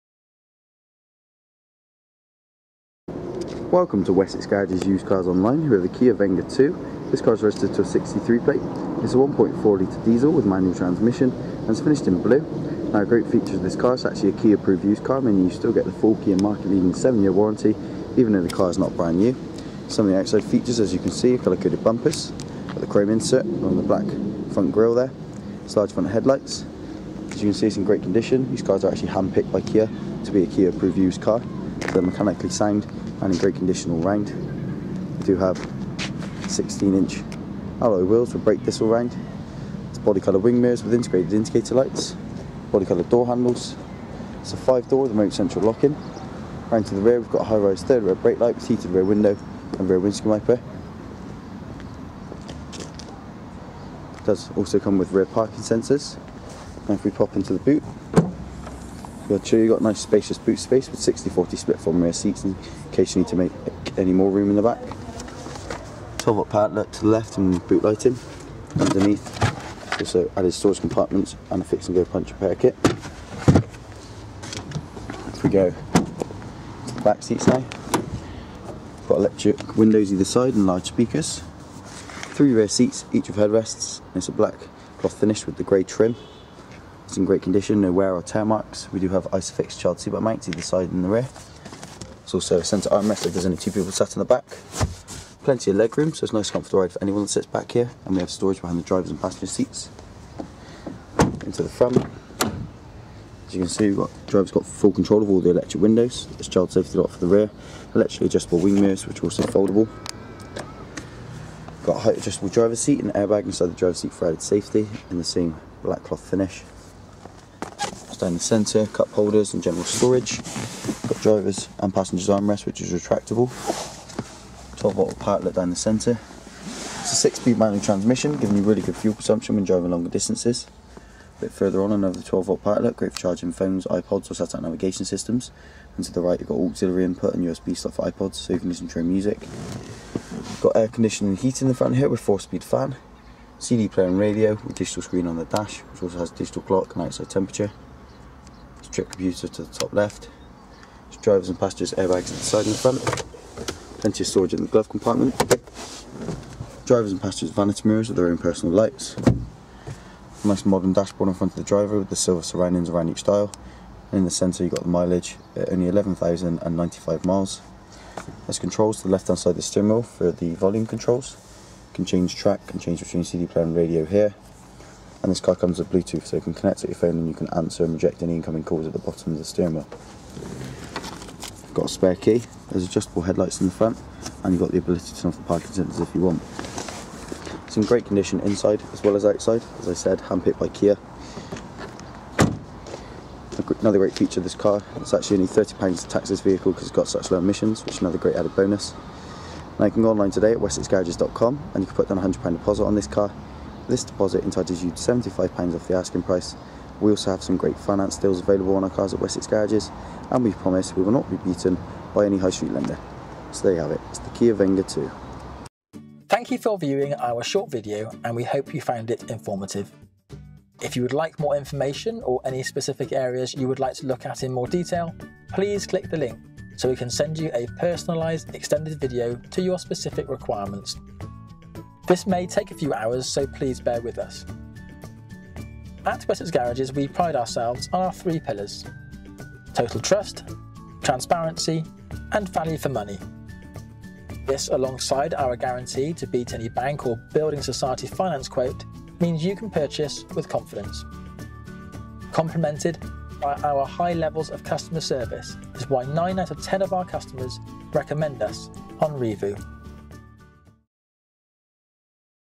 Approved Used Car | Kia Venga 2 (CRDi) | Blue | LJ63OBO | Wessex Garages | Feeder Road | Bristol
The car above is a Kia Venga 2 (CRDi) from Wessex Garages at our showroom at Feeder Road, Bristol.